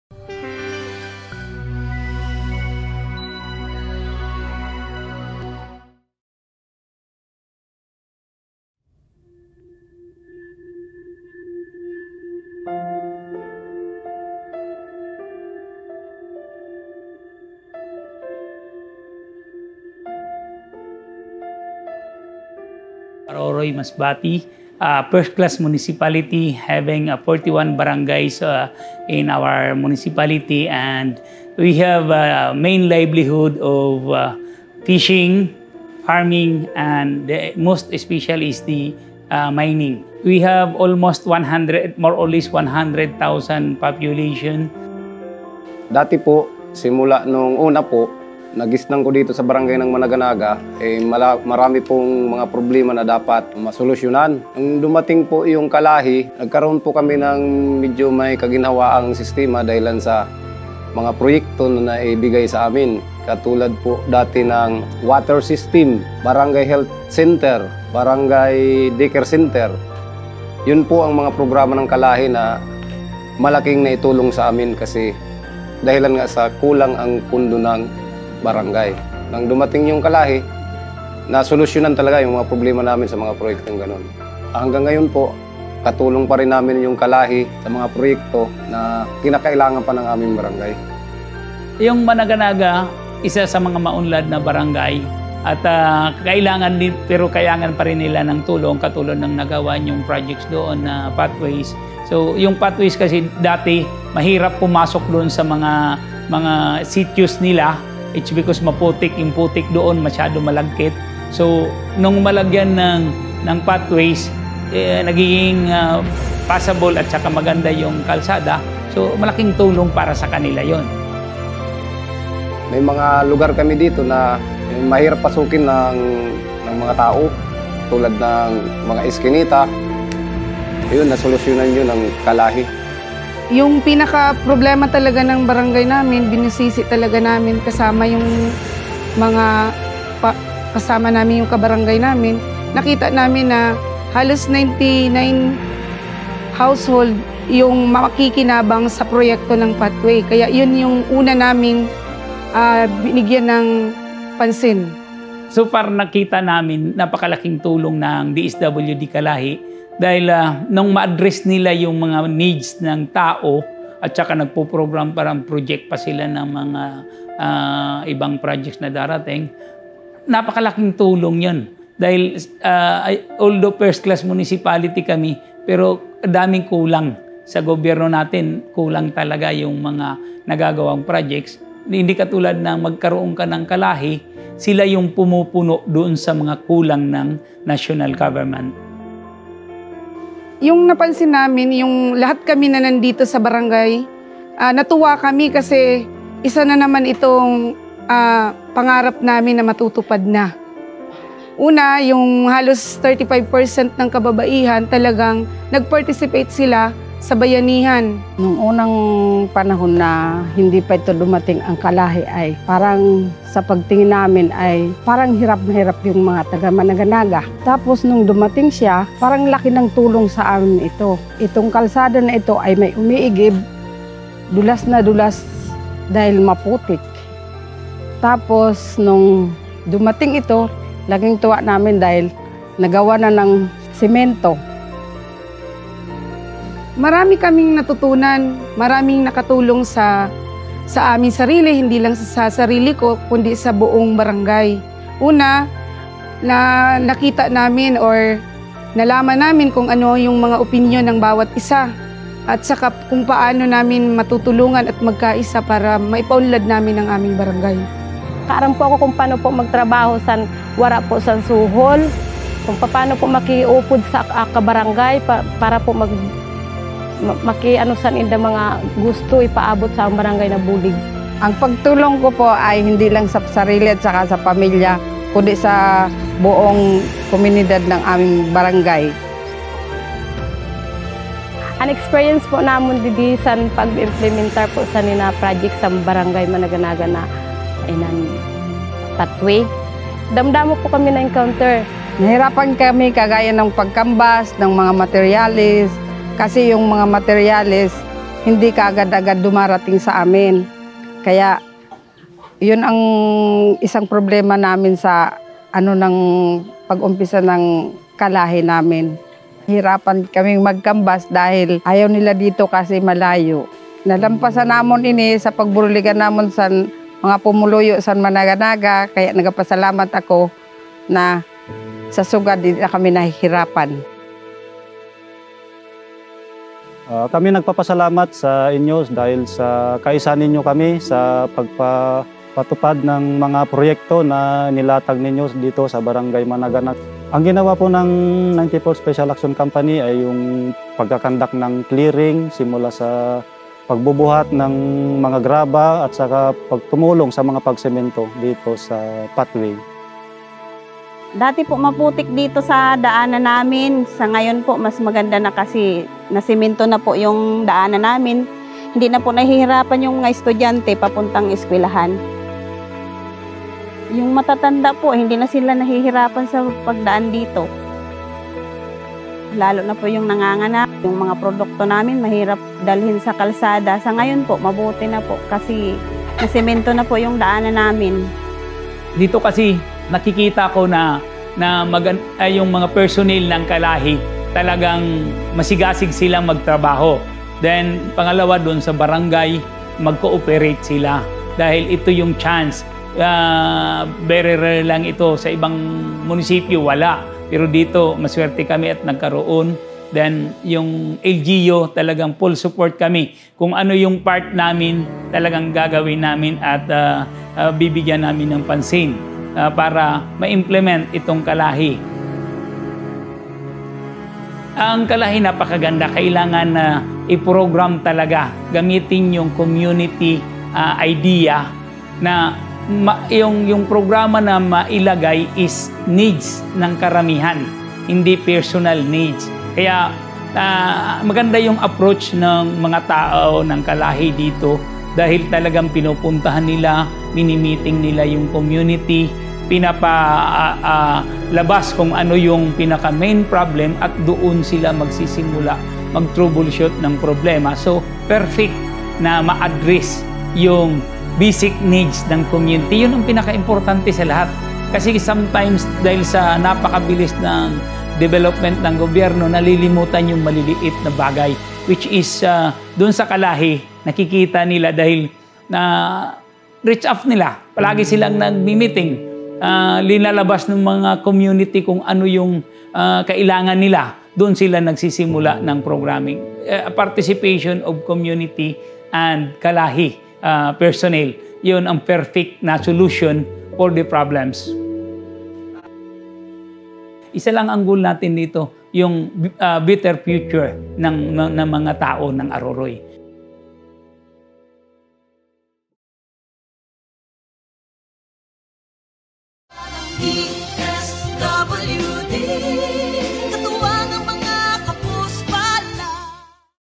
Kwentong Komunidad sa Aroroy, Masbate
Sa likod ng matinding kahirapan, nasilayan ng Barangay Managanaga sa bayan ng Aroroy, Masbate ang pag-asa at tagumpay.

Panoorin ang kanilang kwento kung paano nila naipatupad pathway upang mapadali ang akses ng komunidad sa mga serbisyo.

Maagap at Mapagkalingang Serbisyo!